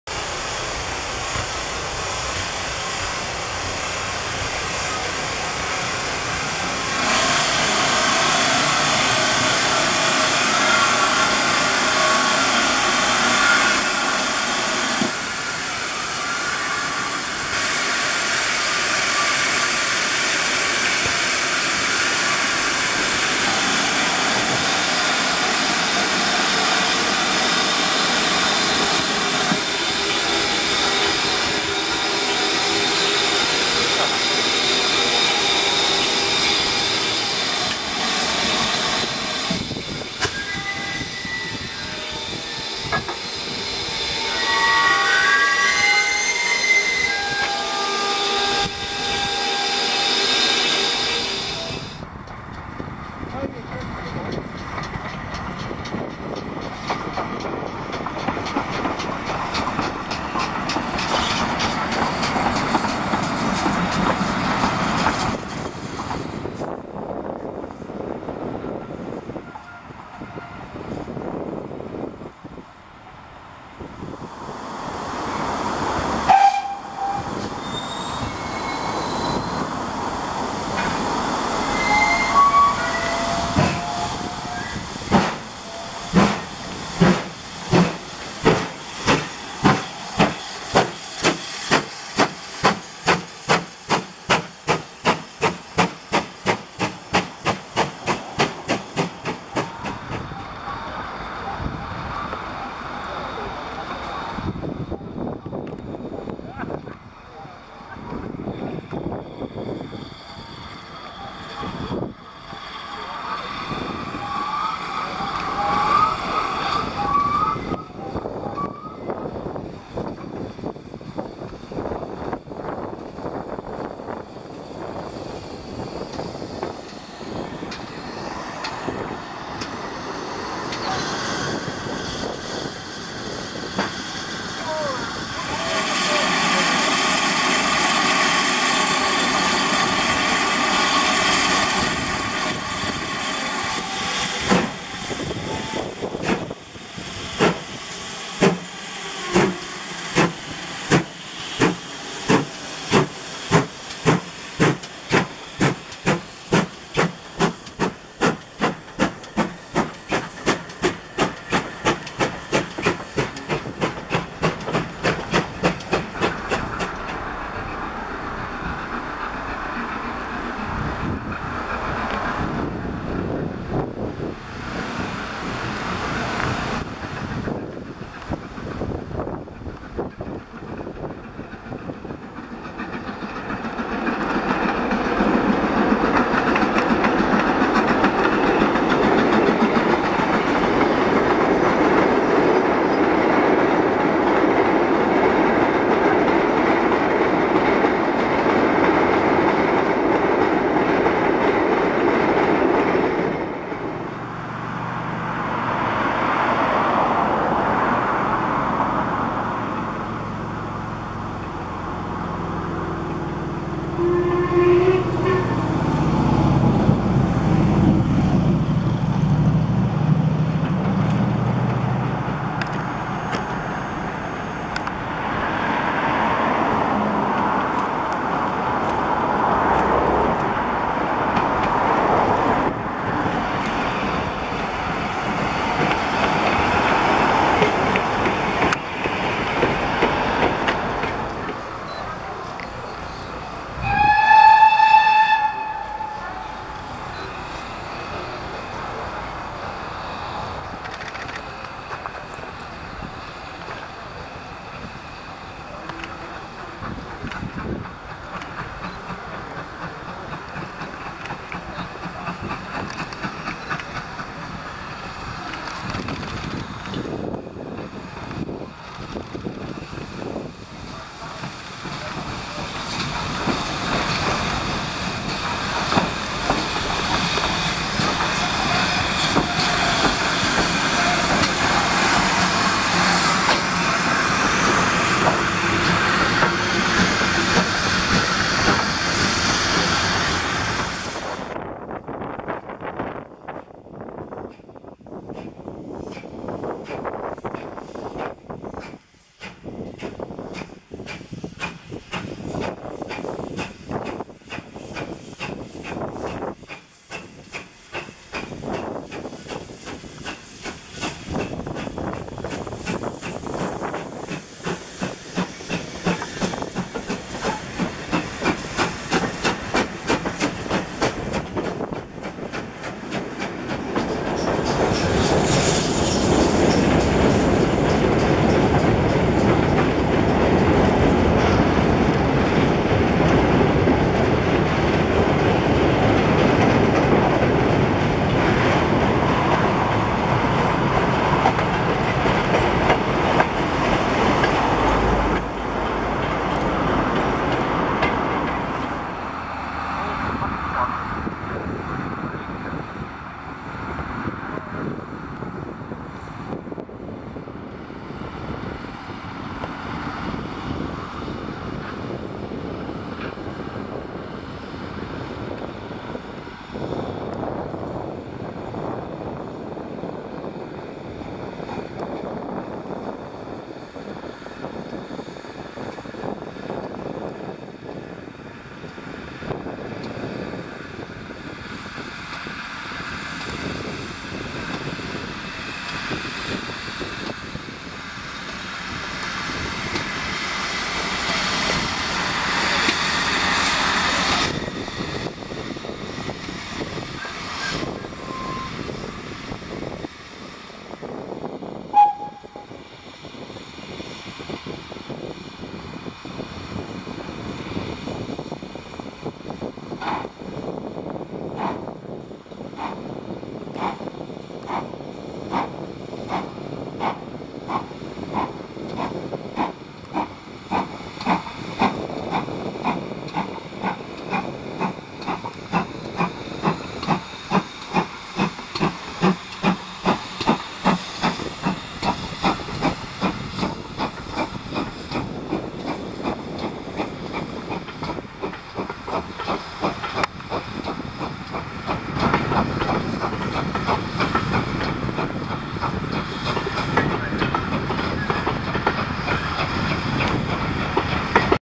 Great Central Rly Steam Gala 1st Oct 21 GWR 3802, LMS Black 5 45305 and GWR 6990 Witherslack Hall
GWR 2884 Class No. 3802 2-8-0 heavy freight
designed Churchward/Collett built Swindon 1938

LMS Stanier Class 5 No. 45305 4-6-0
built Newcastle-on-Tyne 1936

GWR Modified Hall No. 6990 Witherslack Hall
4-6-0 designed Hawksworth built Swindon 1948